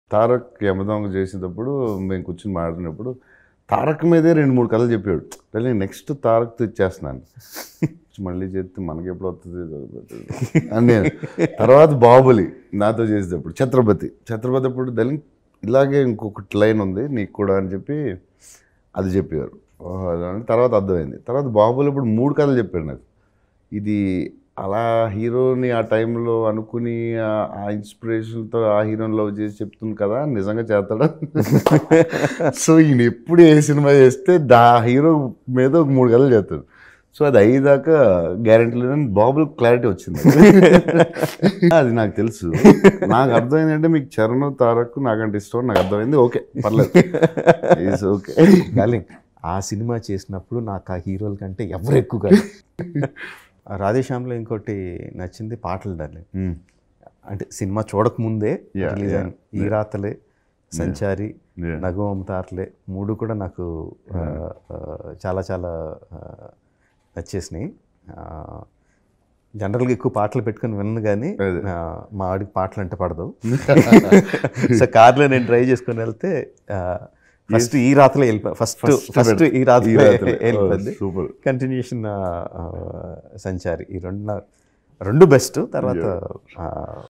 Rajamouli Likes Tarak & Charan More Than Me Says Prabhas | Manastars
Watch, Rajamouli Likes Tarak &# Charan More Than Me Says